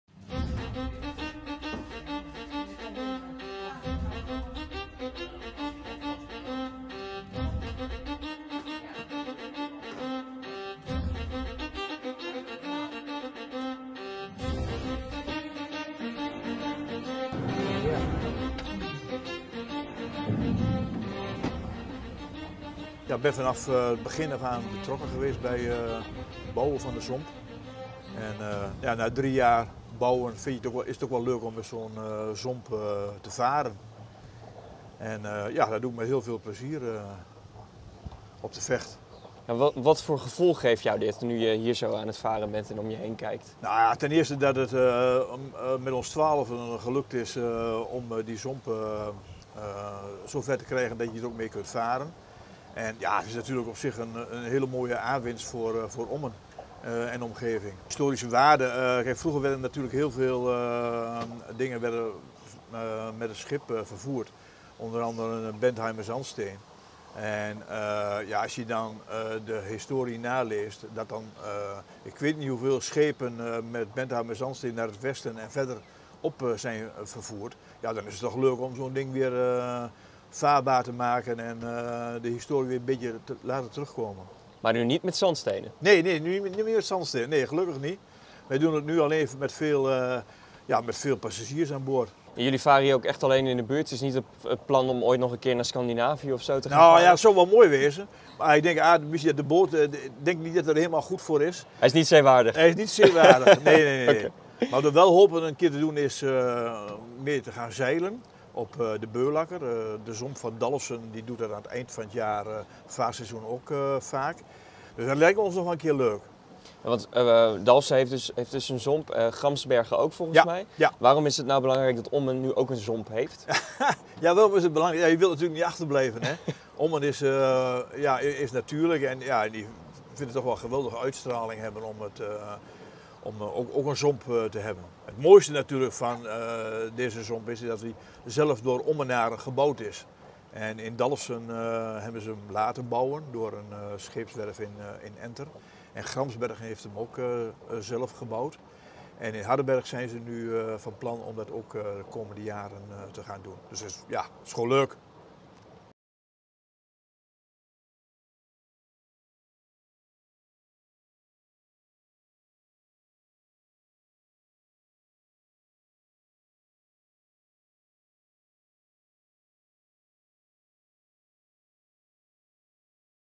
Varen met de zomp over de Vecht is een aanwinst voor Ommen
Van 2020 tot 2023 is er door Ommenaren druk gebouwd aan de Ommer Vechtzomp, maar nu kan het schip eindelijk varen. Langzaam varen over de Vecht in een historisch vaartuig, volgens schipper Hans Hoekman is dit een aanwinst voor Ommen: ''Het is toch leuk om de historie een beetje terug te laten komen.''